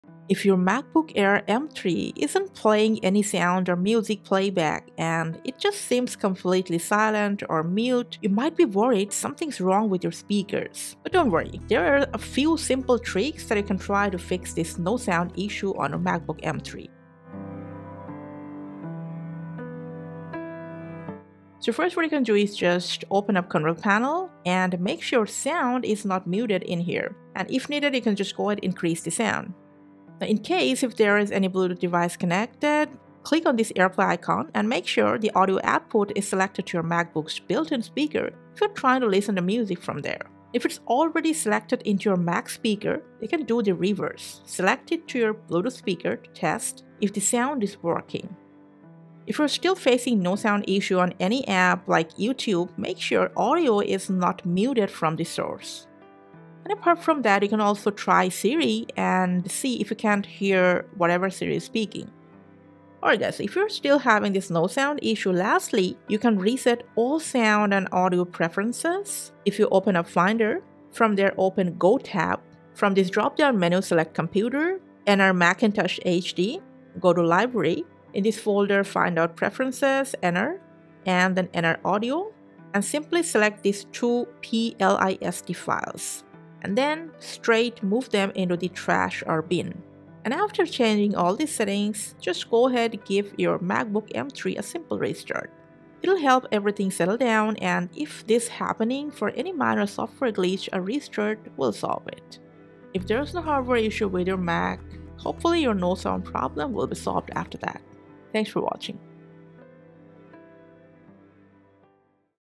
MacBook Air M3: How to Fix Sound Not Working on Mac! - Solve Audio!
Is your MacBook Air with M3 Apple Silicon Chip not playing any sound from the built-in speakers, especially after the latest macOS Sequoia? Are the M3 MacBook Air speakers completely mute, and is no audio output coming out? No worries; Fix369 has a video for you.

Let’s have a look at 3 simple yet effective solutions to resolve Sound not working problems on MacBook Air M3 once and all.

0:00 Intro: M3 MacBook Air Sound Not Working?
0:21 Solution 1: Basic Troubleshooting
0:49 Solution 2: Check Apps and Siri
1:04 Solution 3: Reset All Sound and Audio Preferences